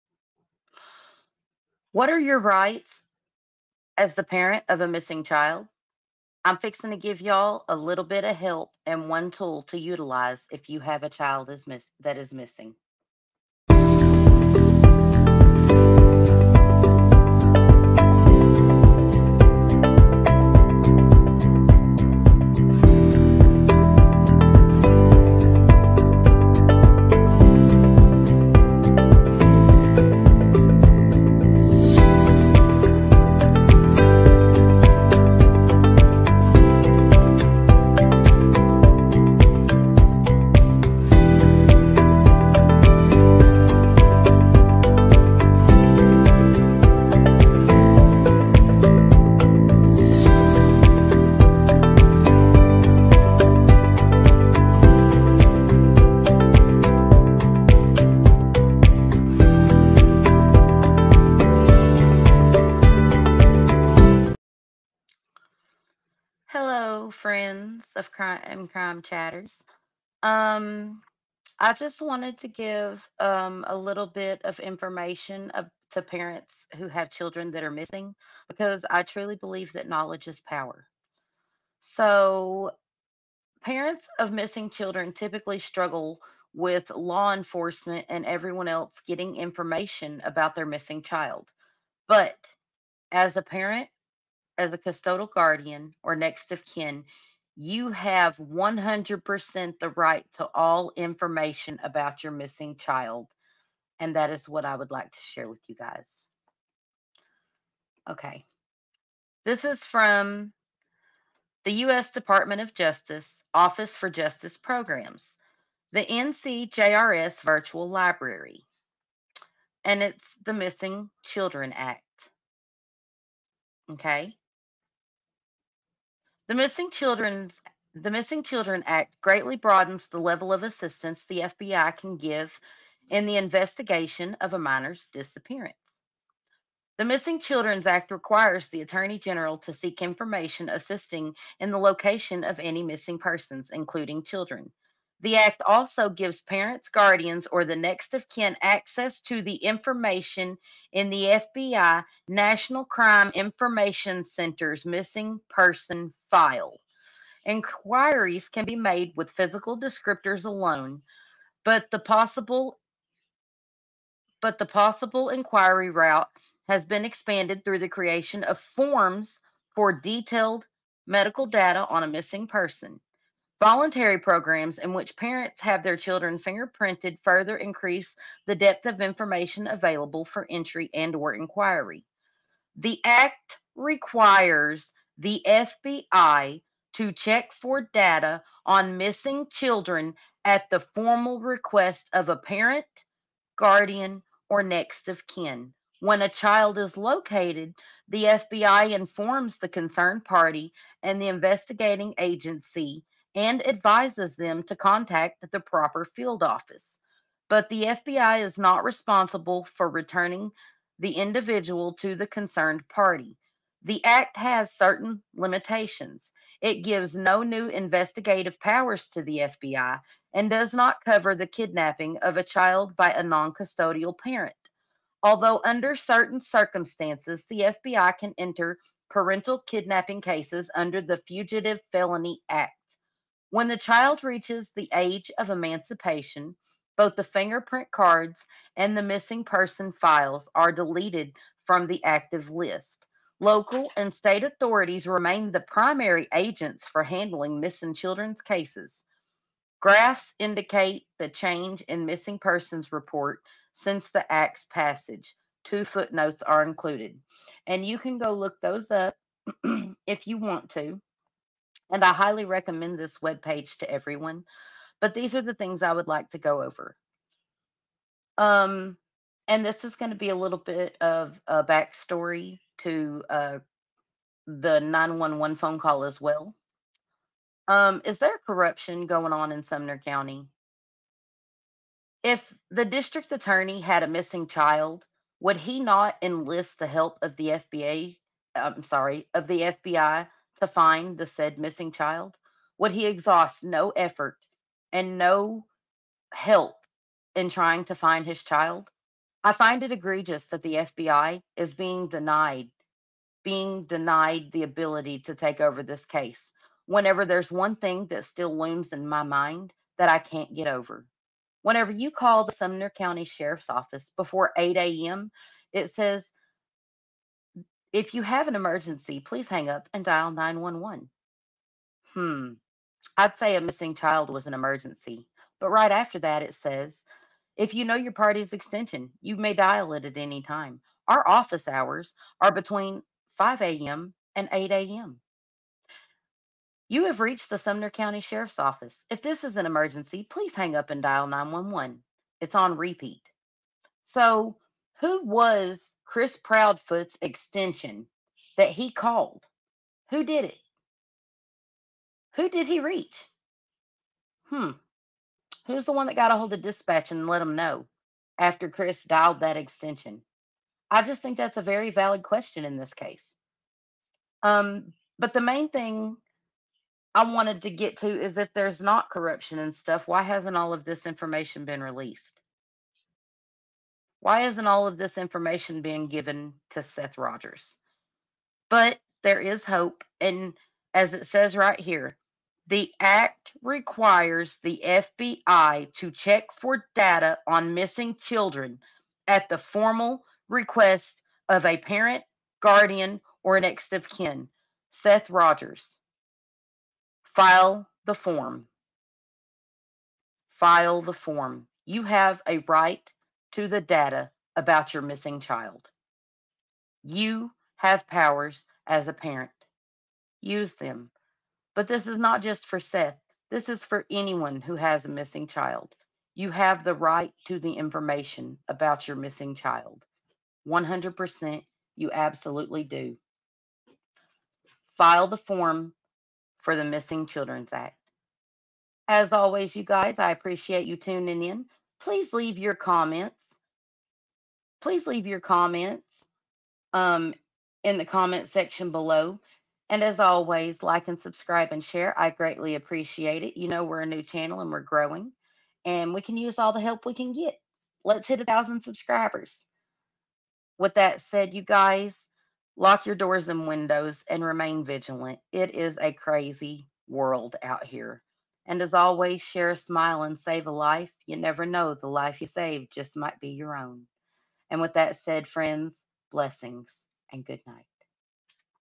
The Right of A Parent of A Missing Child /The Missing Children Act
You as a parent have the right to all files from the FBI on your missing child! Knowledge is Power!